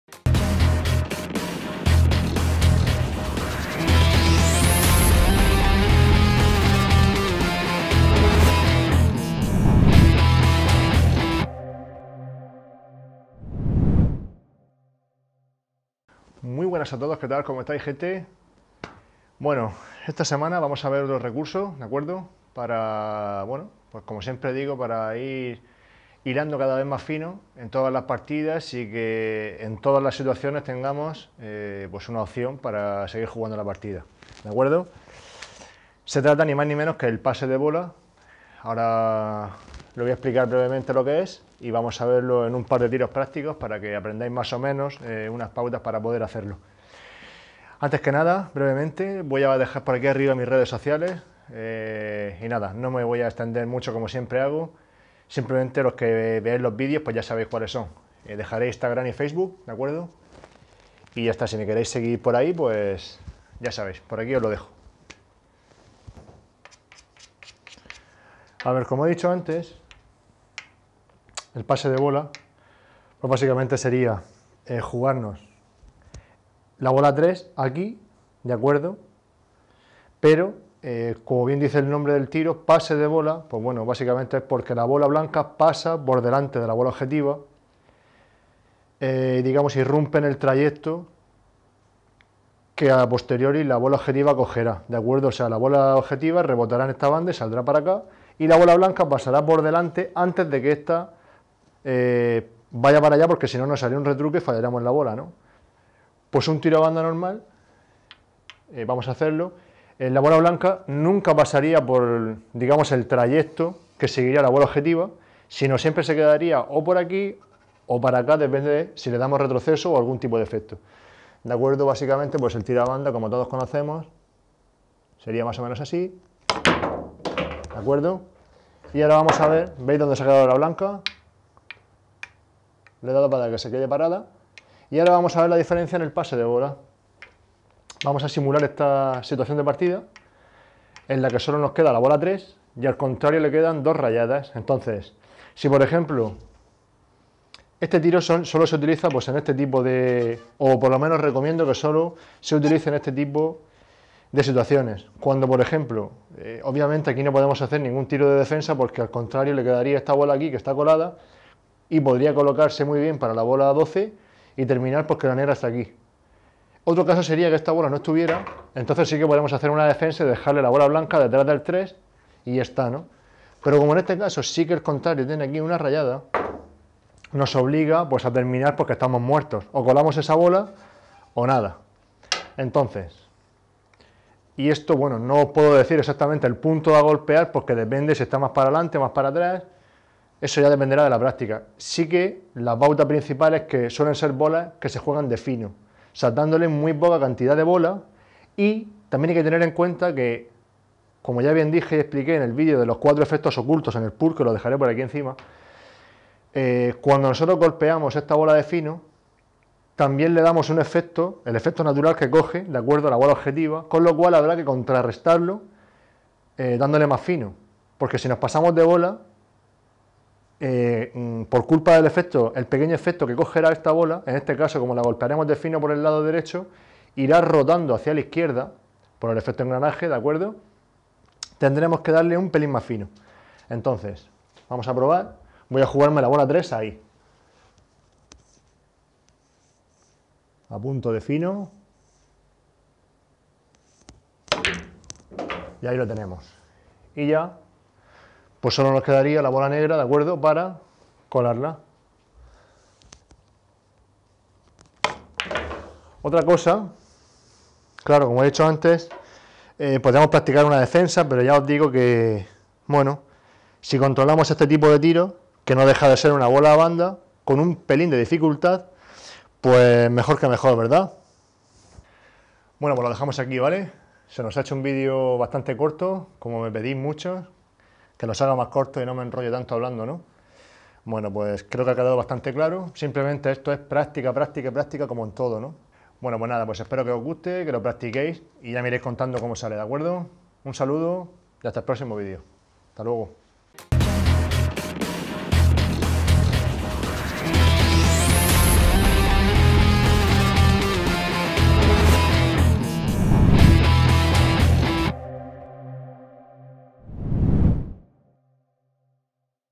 PASE DE BOLA, ¿ QUE ES ?
en este video aprenderemos a hacer un pase de bola, otro buen recurso!!